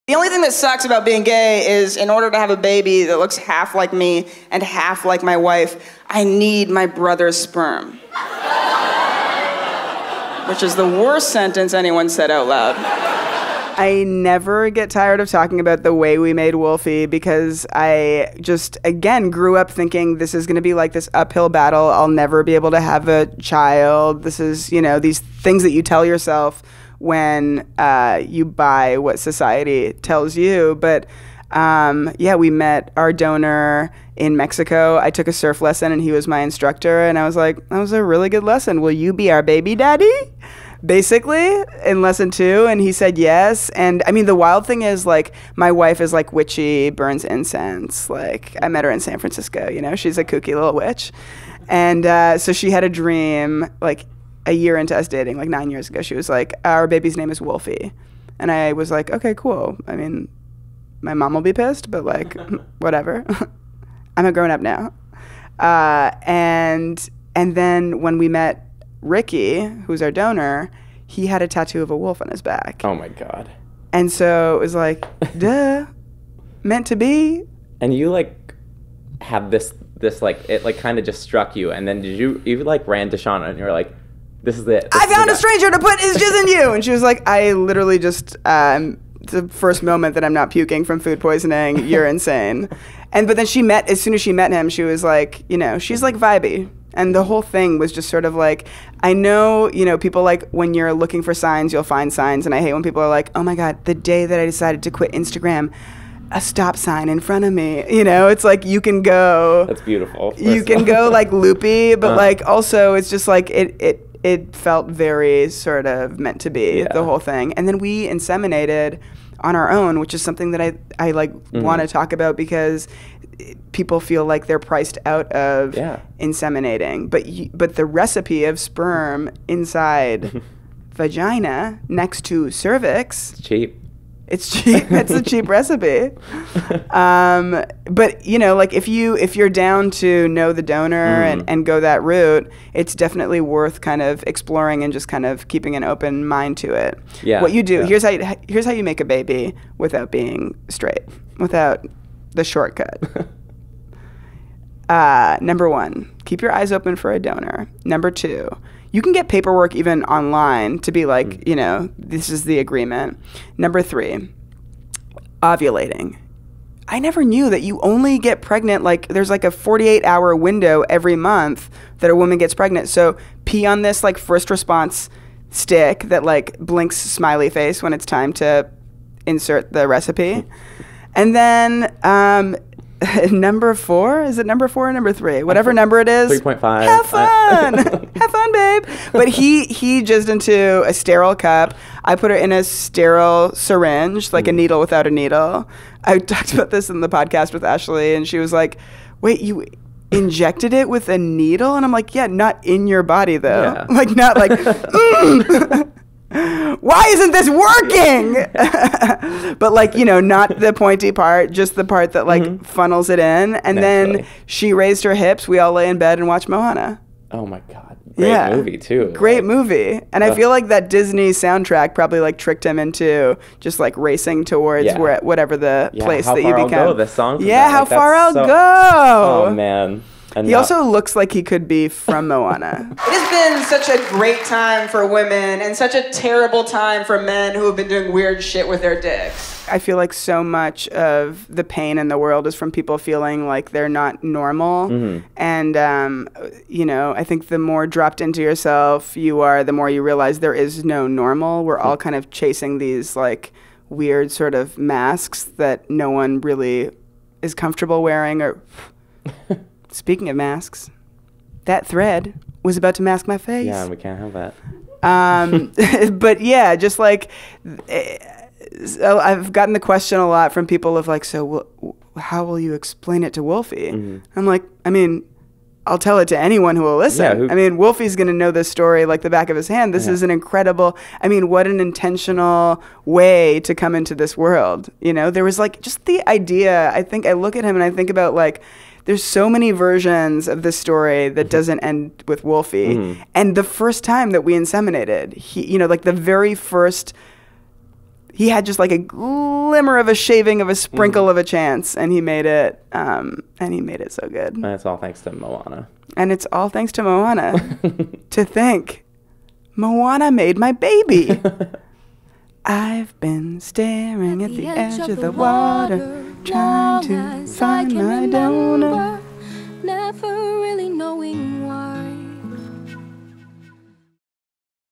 Comedian Sabrina Jalees tells us how to make a baby without "the shortcut"
If you’ve seen Sabrina Jalees’ stand-up or listened to her on a podcast in the past two years, then there’s a chance you’ve heard her tell the amazing story of how she and her wife had their son, Wolfie. It’s a tale of surf instructors, back tattoos, and Moana, and it’s one that the comedian is happy to tell time and again. We recently had the opportunity to sit down with Jalees, and she explained why it’s important that she keeps telling the story and how she hopes to help normalize the process of “independent insemination.” She even went the extra mile and walked us through the four steps to making a baby without “the shortcut.” For more on Sabrina Jalees and her wife’s journey, check out her episode of The Comedy Lineup on Netflix.